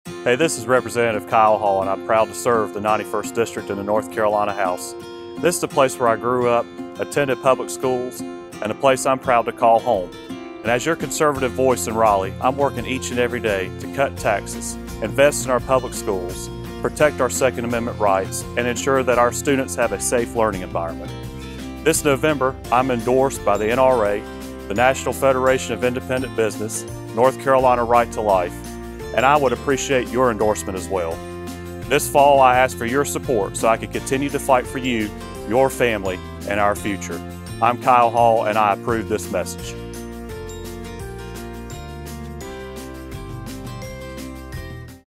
Kyle Hall for NC House
Kyle Hall is the common sense conservative candidate for NC House District 91 - Surry, Stokes and Rockingham Counties.